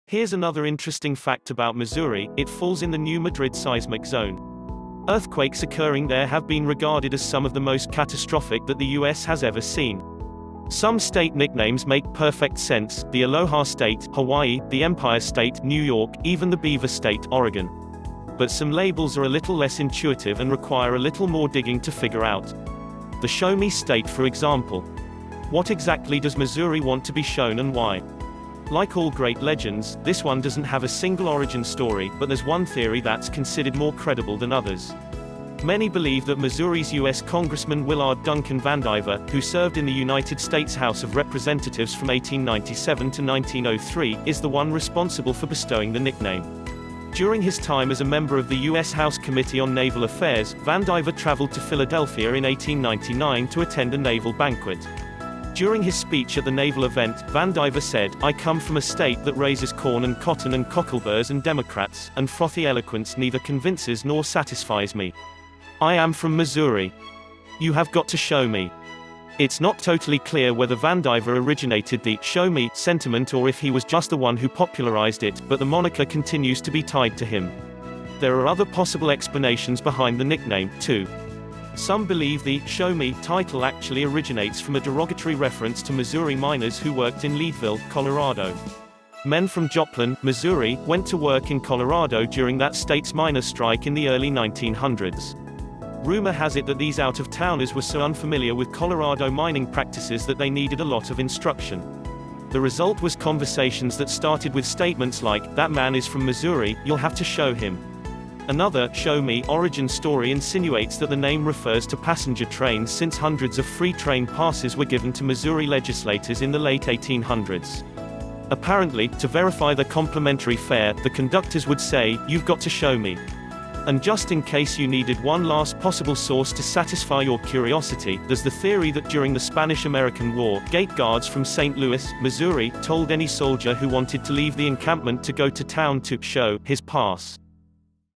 why is missouri called the show me state - 9kink
why is missouri called the show me state Here's another interesting fact about Missouri: It falls in the New Madrid Seismic Zone. Earthquakes occurring there have been regarded as some of the most catastrophic that the U.S. has ever seen.